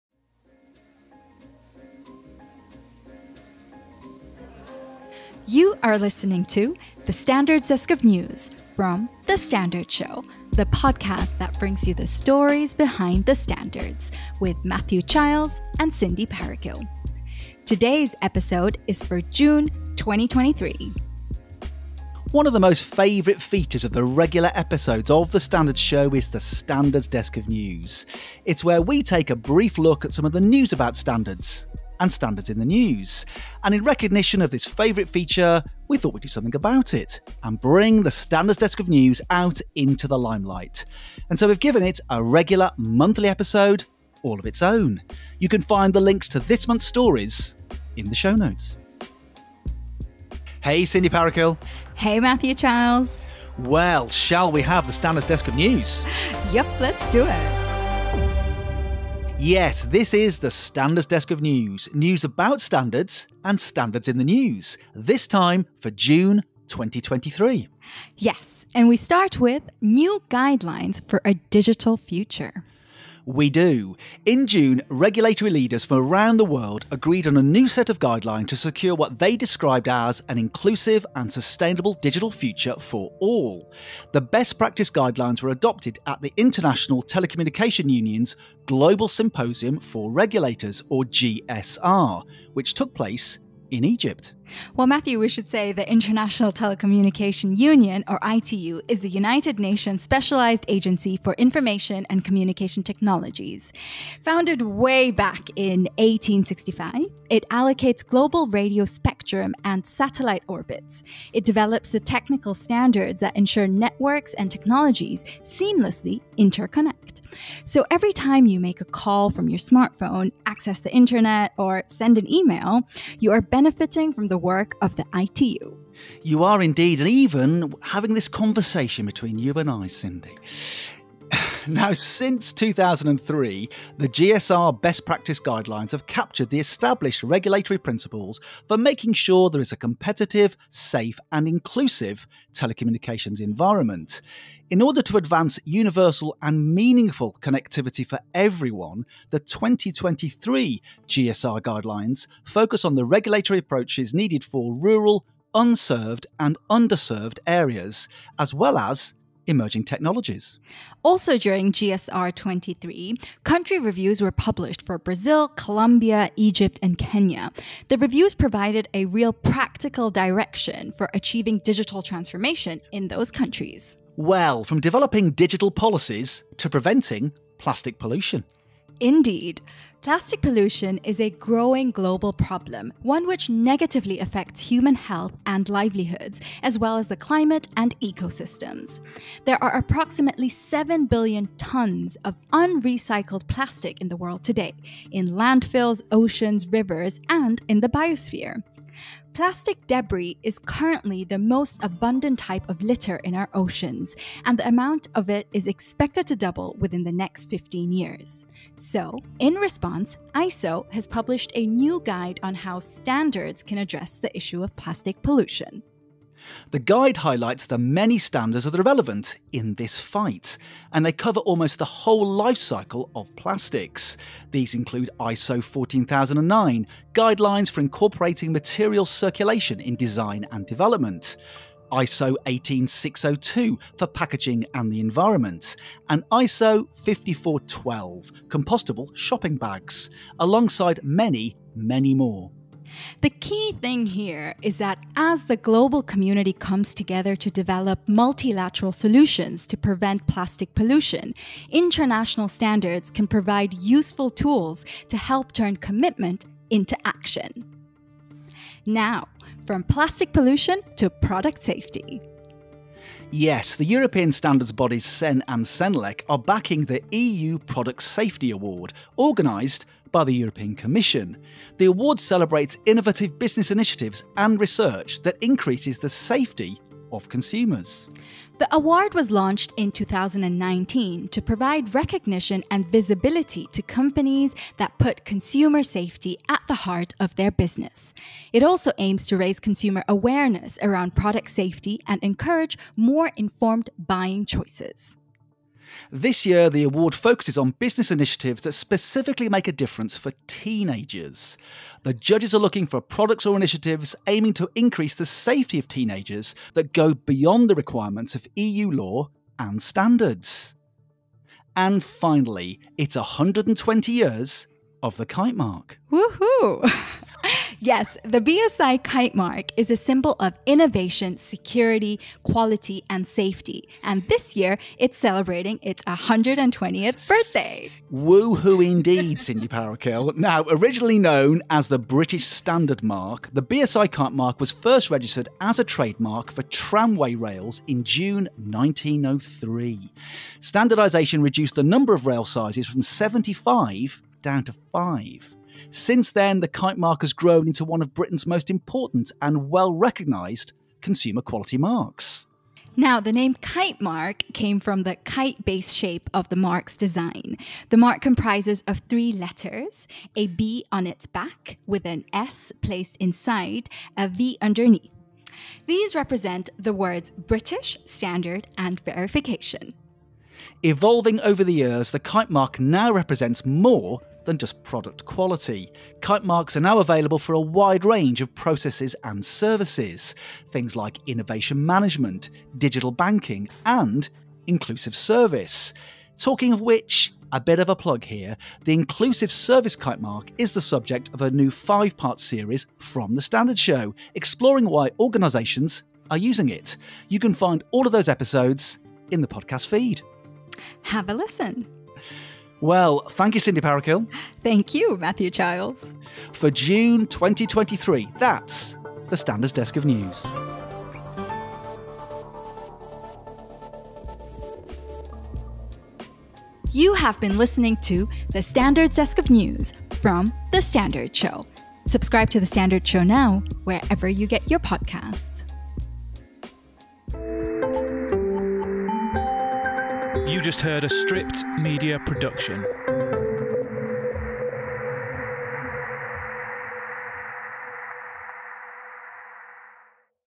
Standards Desk of News | June 2023
One of the favourite features of the regular episodes of The Standards Show is the Standards Desk of News - news about standards and standards in the news.

So, Matthew and Cindy decided to bring the SDON out into the limelight. And to give it a regular monthly episode all of its own.

The Standards Desk of News for June 2023:

1. ITU guidelines on creating a digital future for all
2. ISO guide on standards can help prevent plastic pollution
3. European product safety award on the needs of young people
4. Celebrating 120 years of the BSI Kitemark

Suggest a story for The Standards Desk of News

Get in touch with The Standards Show